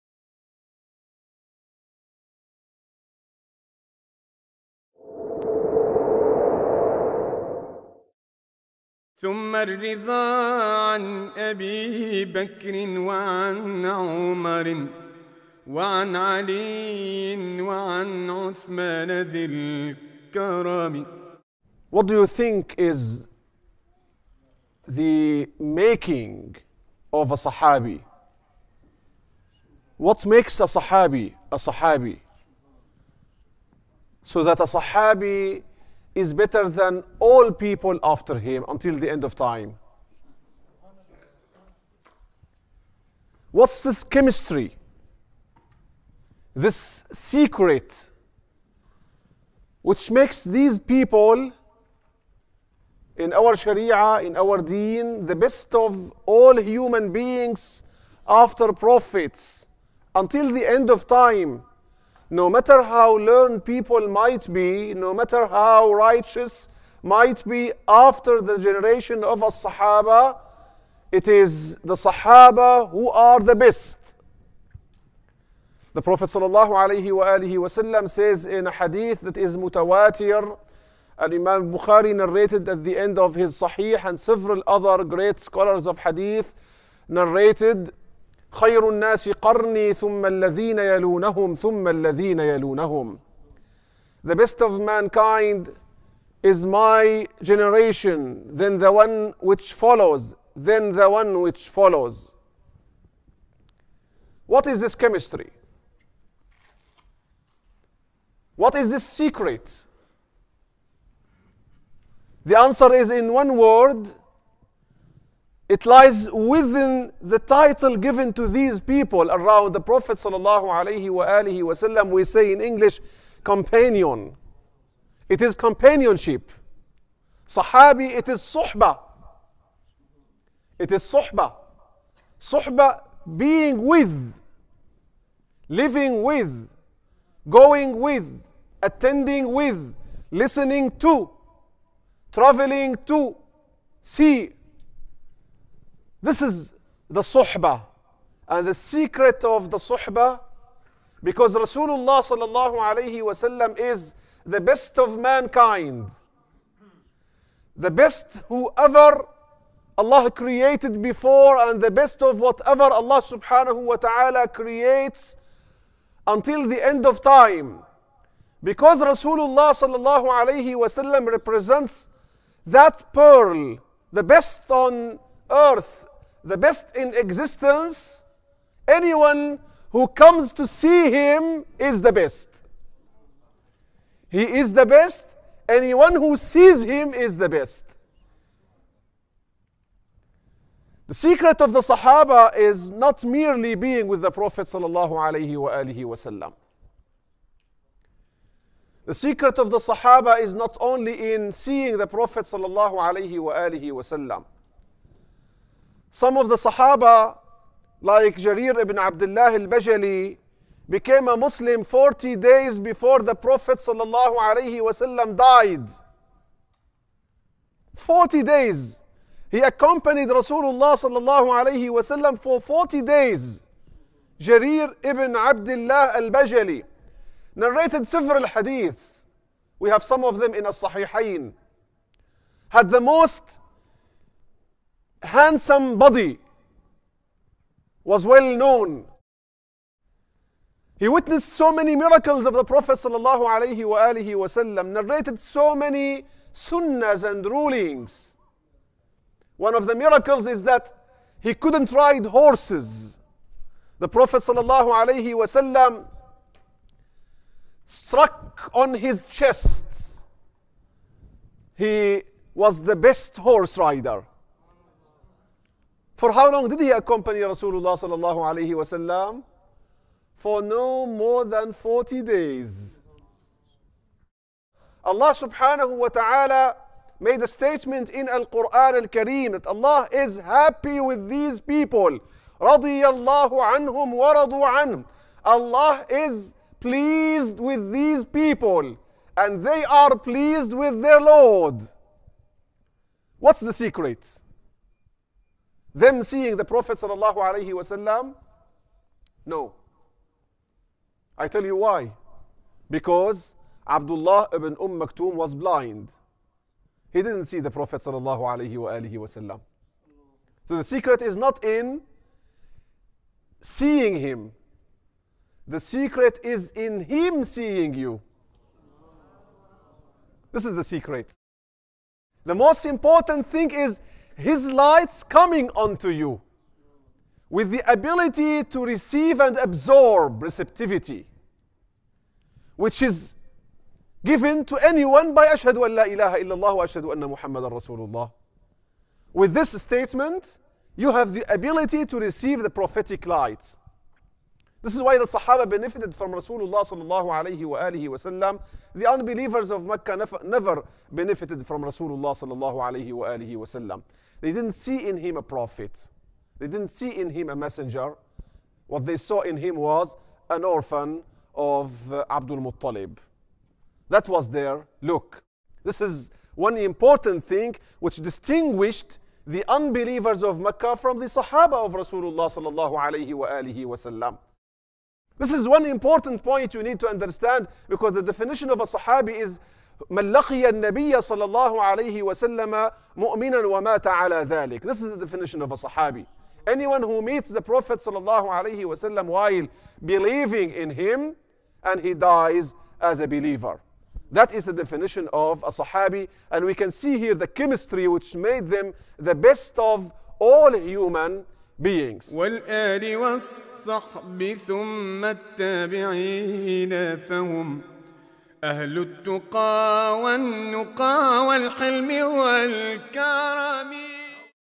Shejh Muhammed El-Ja'kubi - "Sekreti i shokëve të Resulullahut (salallahu 'alejhi ves-selem)"
-Ligjërues: Shejh Muhammed El-Ja'kubi, Allahu e ruajttë!
-Tema: "Sekreti i shokëve të Resulullahut (salallahu 'alejhi ves-selem)".
-Realizoi në shqip: kanali në Youtube "Vëllau Medh'hebist".